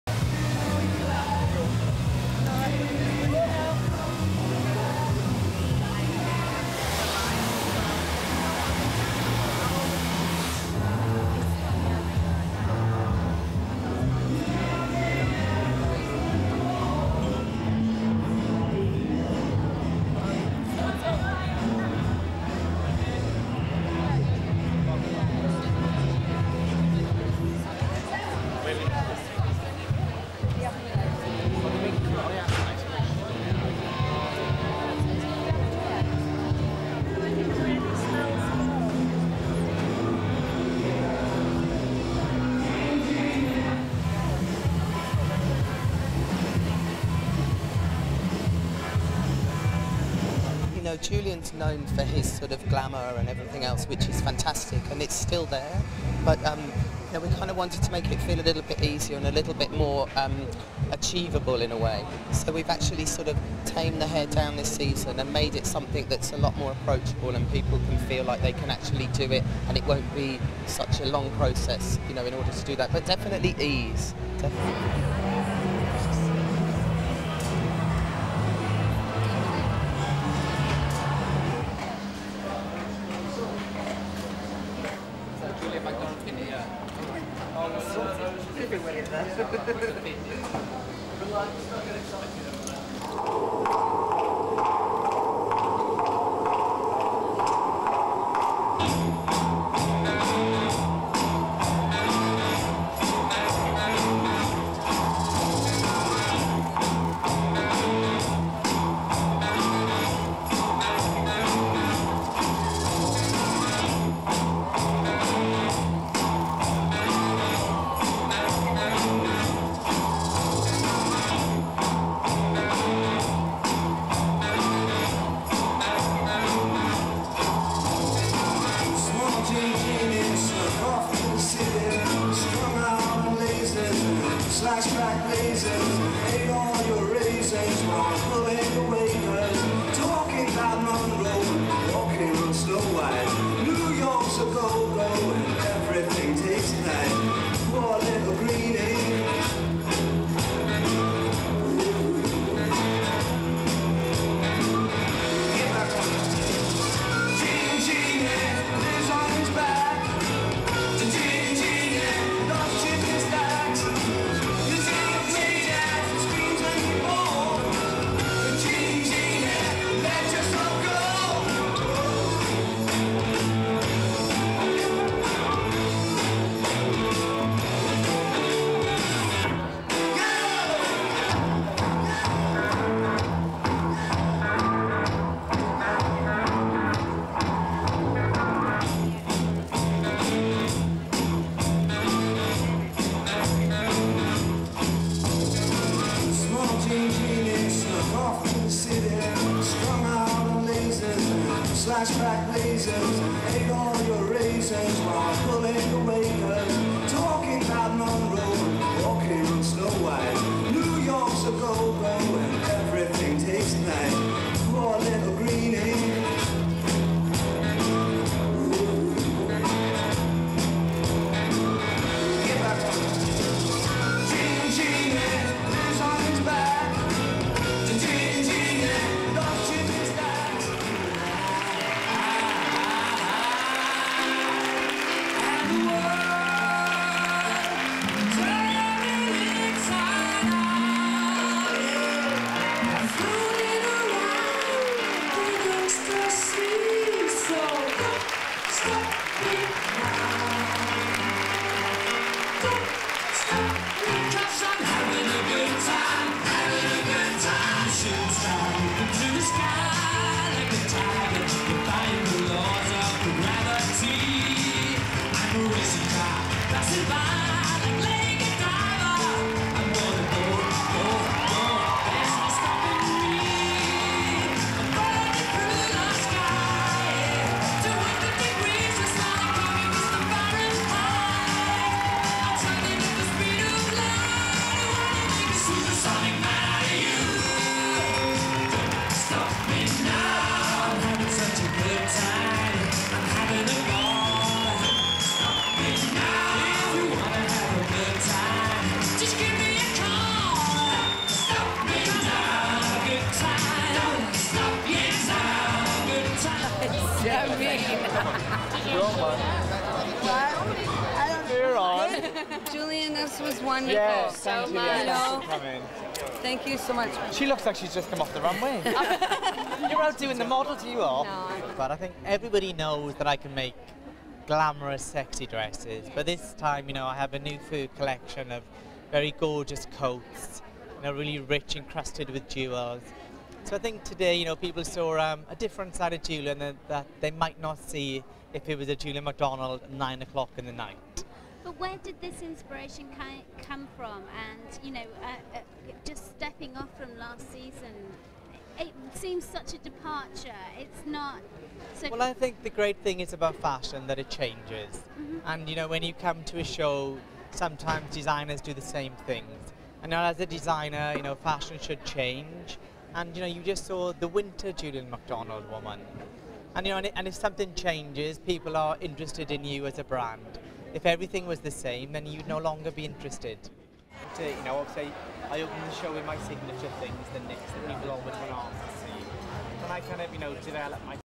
Haute couture meets movie star glamour at London Fashion Week
1. Various backstage of models
2. SOUNDBITE (English) Neil Moodie- hairdresser:
"Julien's known for his sort of glamour and everything else, which is fantastic and it's still there but we kind of wanted to make it feel a little bit easy and a little bit more achievable in a way, so we've actually sort of tamed the hair down this season and made it something that's a lot more approachable and people can feel can feel like they can actually do it and it won't be such a long process, you know in order to do that. But definitely, ease."
3. Various backstage
4. Catwalk Show with various celebrities front row (Juliette Lewis, Rachel Hunter, Sophie Ellis Bextor, Jools Oliver)
5. Grand Finale with Julien MacDonald and Naomi Campbell
6. Various after party.
7. SOUNDBITE (English) Julien MacDonald:
"I think everybody knows that I can make glamorous, sexy dresses but this time I have a new fur collection of very gorgeous coats and they're really rich, encrusted with jewels. So I think today, people saw a different side of Julien that they might not see if it was a Julien MacDonald, nine o'clock in the night."
(Q: But where did this inspiration come from?)
"Well I think the great thing about fashion is that it changes and you know, when you come to a show, sometimes designers do the same things and as a designer, fashion should change and you know, you just saw the winter Julien MacDonald woman. And if something changes, people are interested in you as a brand. If everything was the same, then you'd no longer be interested."
8. Cutaway after show party.

HIGH GLAMOUR ON MACDONALD CATWALK

Haute couture met red carpet glamour as London Fashion Week kicked off with movie star style on Sunday (13th February 2005).

Flamboyant Welsh designer Julien Macdonald's autumn/winter offerings were the highlight of the first day of London fashion shows, revealing a collection featuring fur and crystals.

knit sweaters shone with sequins, beads and their metallic threads. Sweaters and V- necked vests were worn tight and paired with frilly blouses.

In defiance of anti-fur activists he sent one of his favourite models, Elizabeth Jagger, down the catwalk with a fox pelt over her shoulder.

But the dress that stole Macdonald's show had already had its first outing - on the British Academy Film Awards red carpet the previous evening.

Hollywood actress Juliette Lewis wore the Bordeaux red silk georgette gown with a bird of paradise feather train to Saturday's glitzy awards ceremony at London's Leicester Square.

The gown, shimmering in Swarovski crystals and valued at $46,708, was a signature piece of Macdonald's catwalk show.

Other celebrities who joined Lewis, dressed in a Macdonald-designed three-piece suit, in the audience at his show included model-actress Rachel Hunter, singer Sophie Ellis-Bextor, underwear model and reality television star Caprice, celebrity chef Jamie Oliver's wife Jools and artist Tracey Emin.